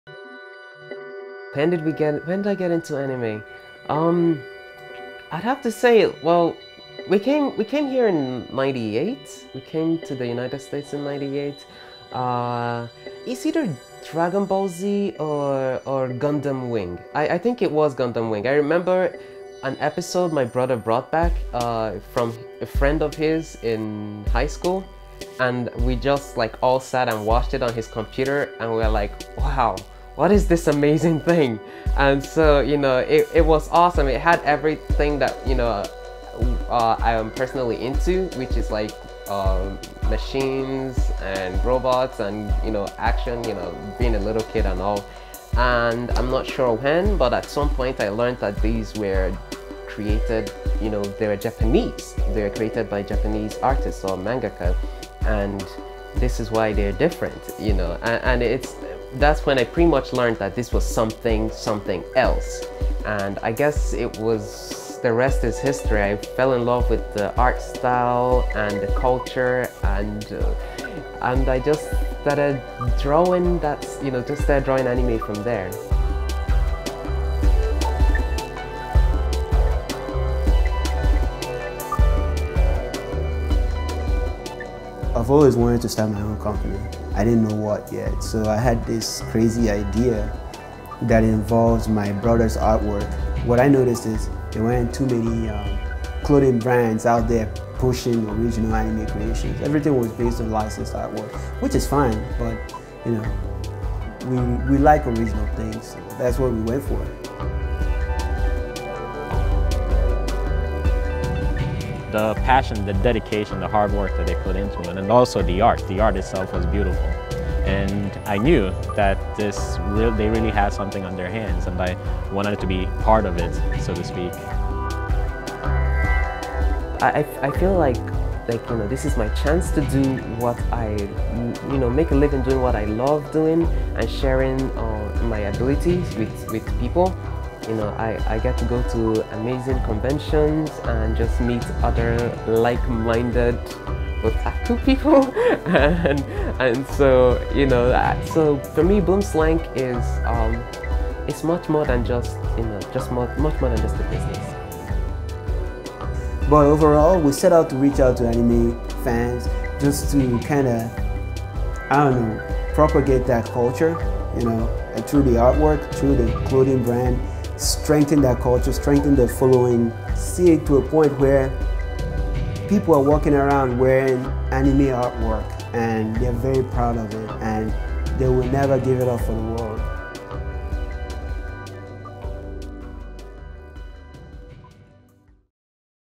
Boomslank
The word Boomslank is completely made up. The idea behind Boomslank was born out of a strong need to do what makes one happy in life. We started Boomslank because we noticed a lot of the anime tees out there relied heavily on already licensed artwork. There was nothing original out there. So we set out to create an anime brand that was strictly based on original artwork.

So far things are going great and we are really enjoying the journey of trying to follow our dream. And believe me, we've only just begun our journey.